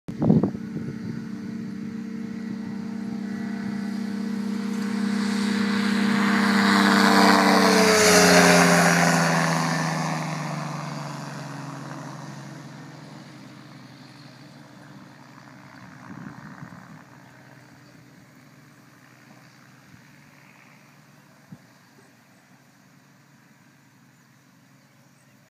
Fox V6 em Mangaratiba
Testando o V6 do Cmte. Marlboro em Aero Rural Bom Jardim (SDCZ) Mangaratiba